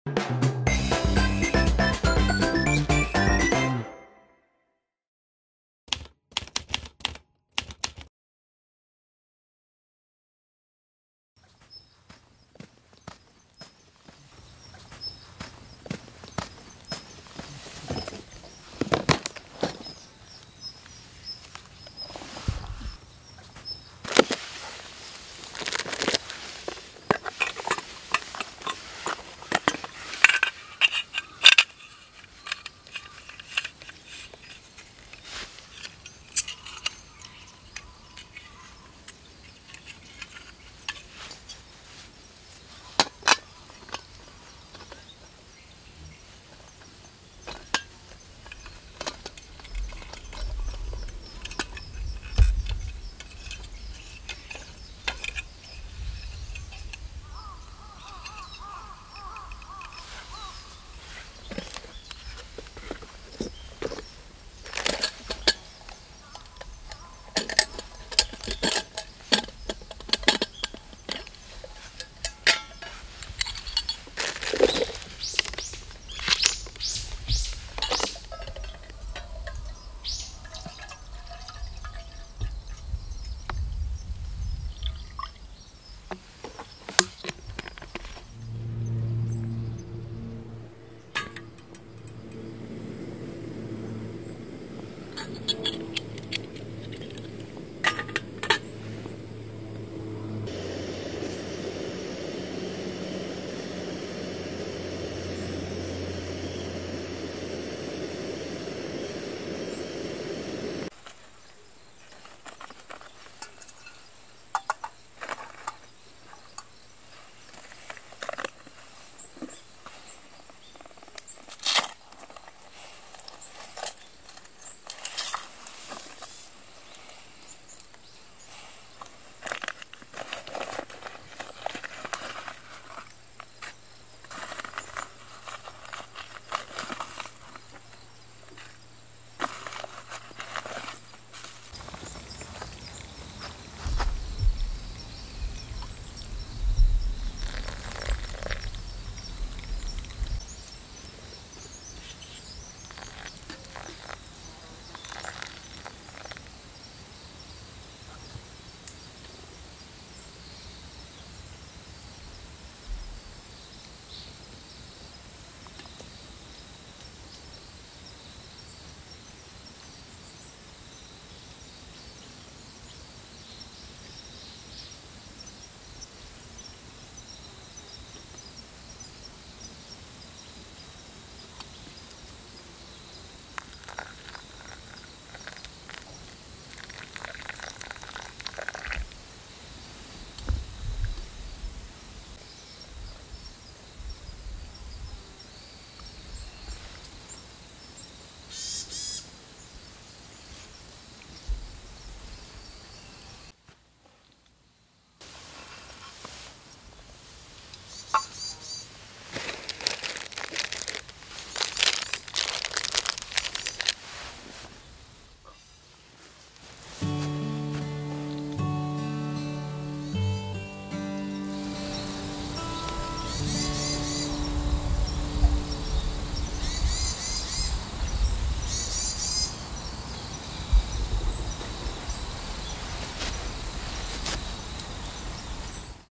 エフエムサンアウトドア部Campの会「アウトドア飯２」
エフエムサンアウトドア部Campの会の２回目。今回からいよいよ調理スタートです。といっても今回は超簡単。初心者の導入にはぴったりの「コーヒー」です。外でお湯を沸かすだけでもテンション上がりますよ♪

先生：エフエムサン専属？師匠　作花勉さん